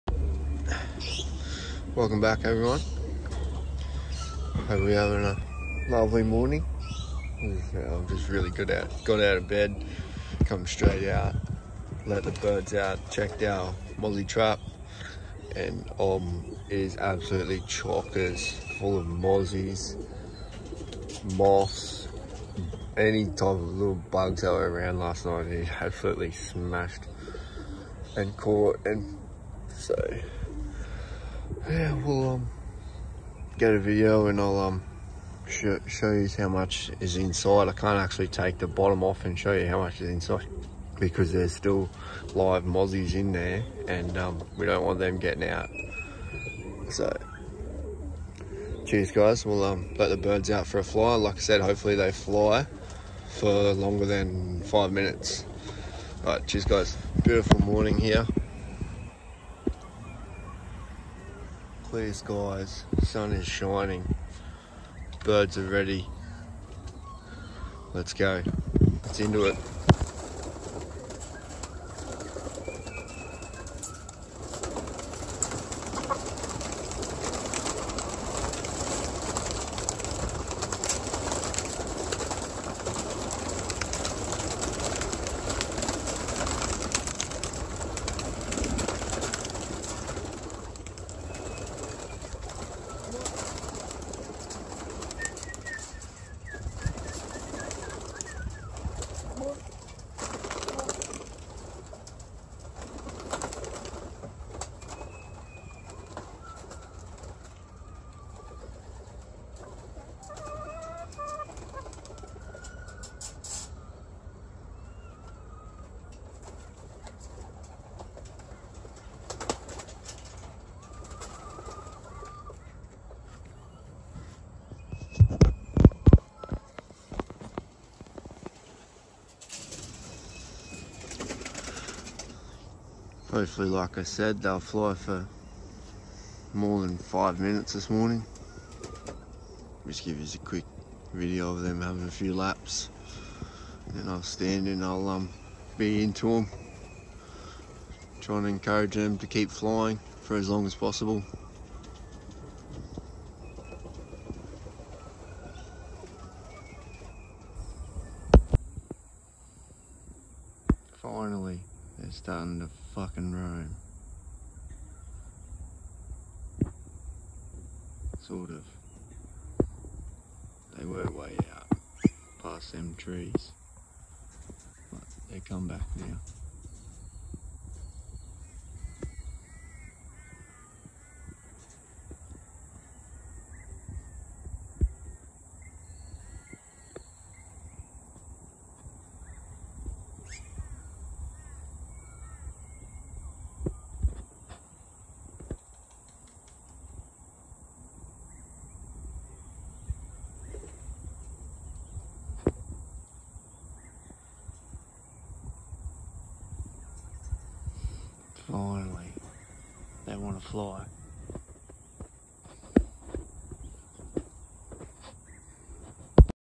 Pigeons - The Birds Working Nicely!!
Thursday morning Excerise for the birds will get them out again this afternoon for the afternoon, see how they go in the afternoon, pretty happy with them atm,